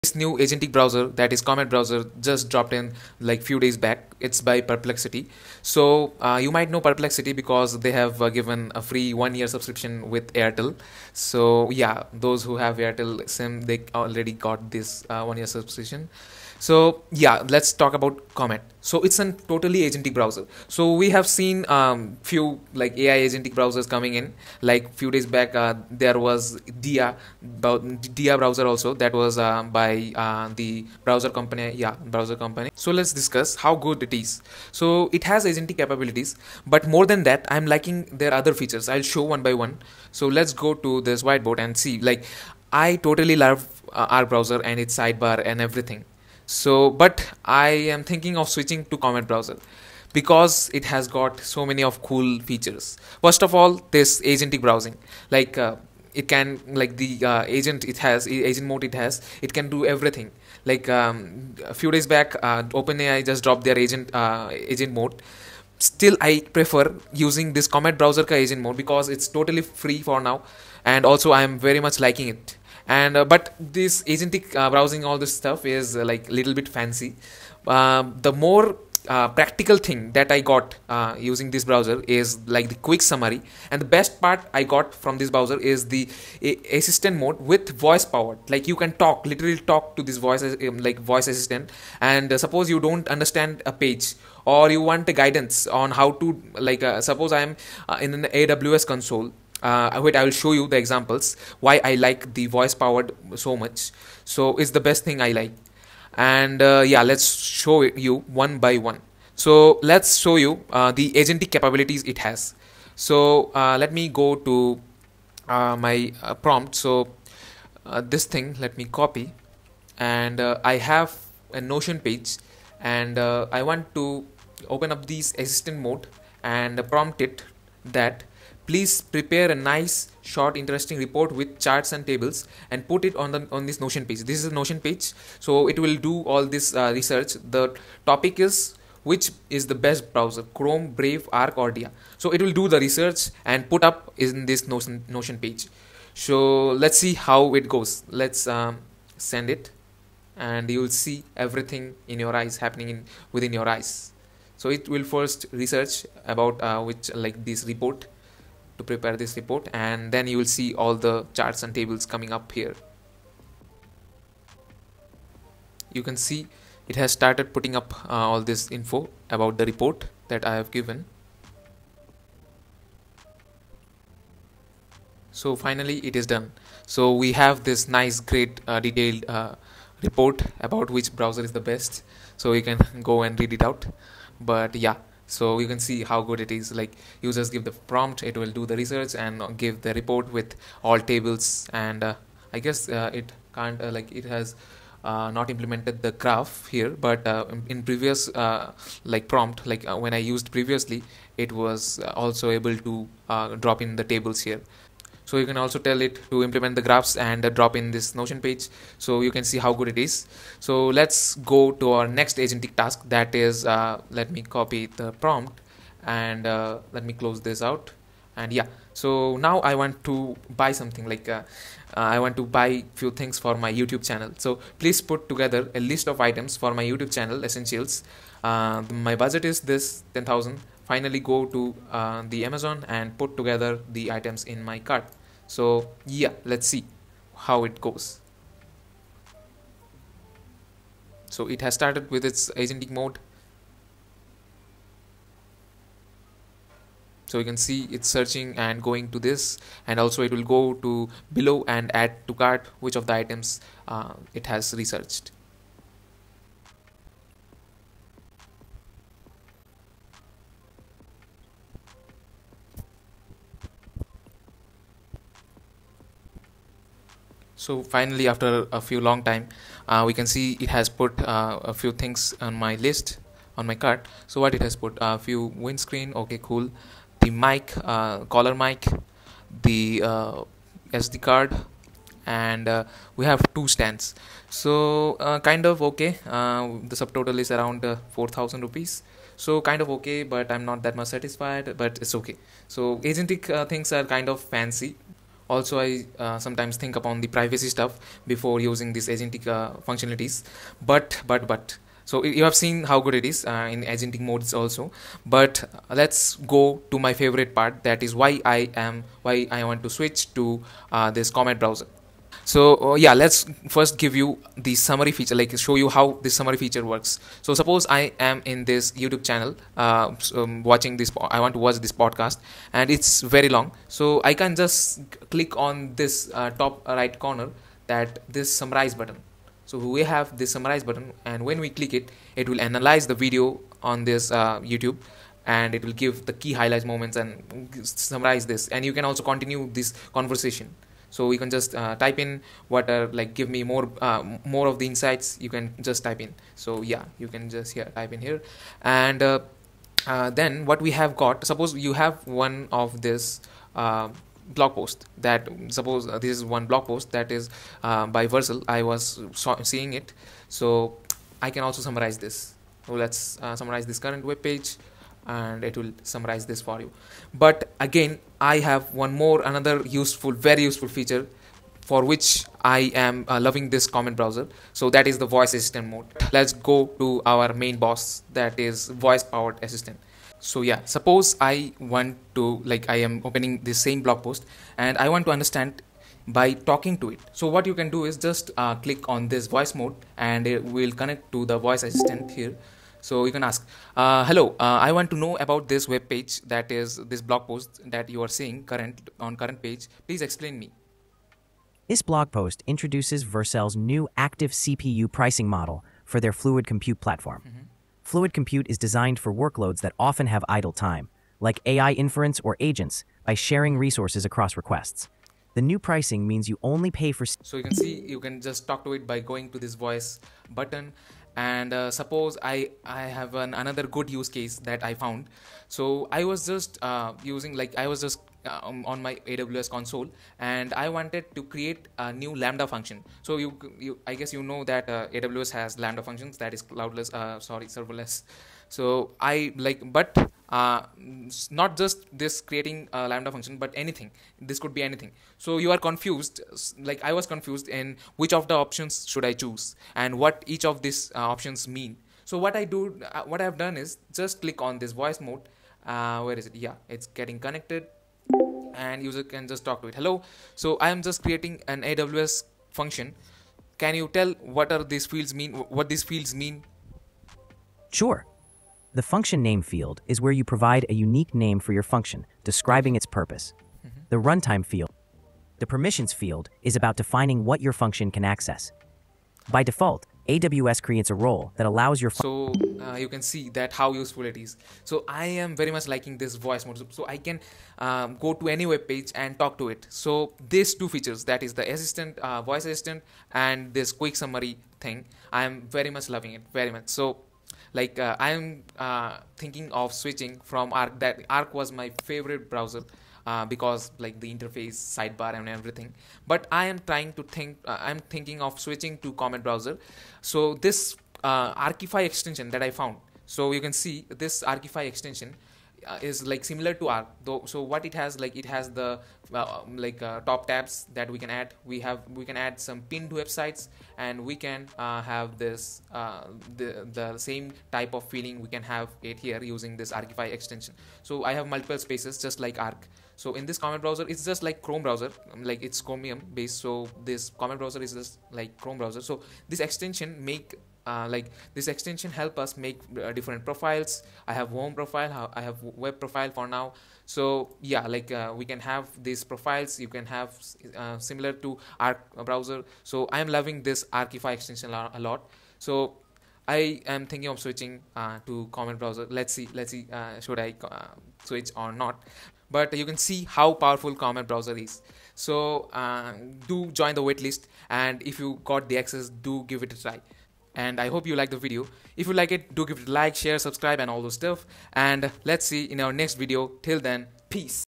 Perplexity's Comet: THIS AI NATIVE BROWSER just feels so better than Chrome, Arc, Dia 🔥
Ready to see what a truly “agentic” browser looks like? Meet Comet by Perplexity AI—a free, AI-powered browser that researches, writes, shops, summarizes, and even talks back in real time. In this video I put Comet through its paces: auto-building a sleek Notion report, filling an Amazon cart on voice command, turning hour-long YouTube videos and chunky blog posts into snack-size takeaways, and giving Arc-style vibes with clever tab spaces. If you’ve ever wished Chrome, Brave or Arc could actually do the work for you, watch till the end and decide whether Comet deserves your default-browser crown.